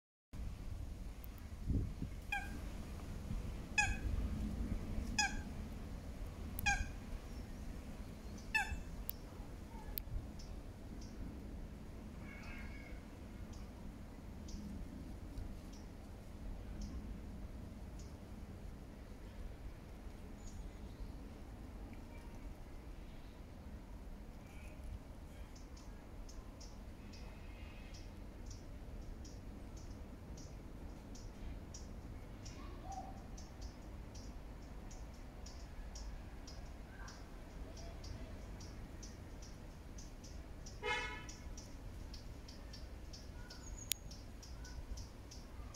Pretty damselflies.
Pretty damselflies in Regent's Park London. August 2024.